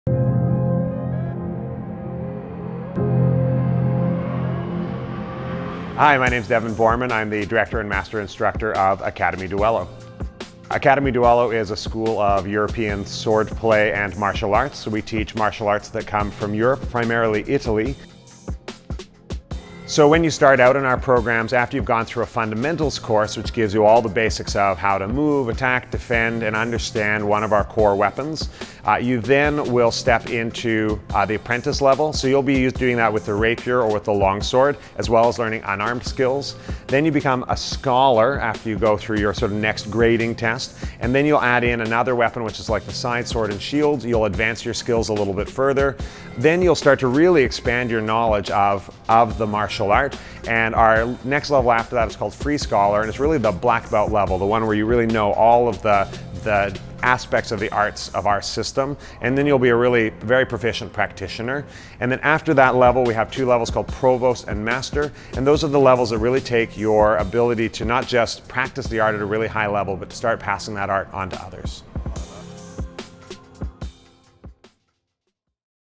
Academie Duello - Levels and Courses
Director of Academie Duello - Maestro Devon Boorman, talks about different types of courses and
the rank system.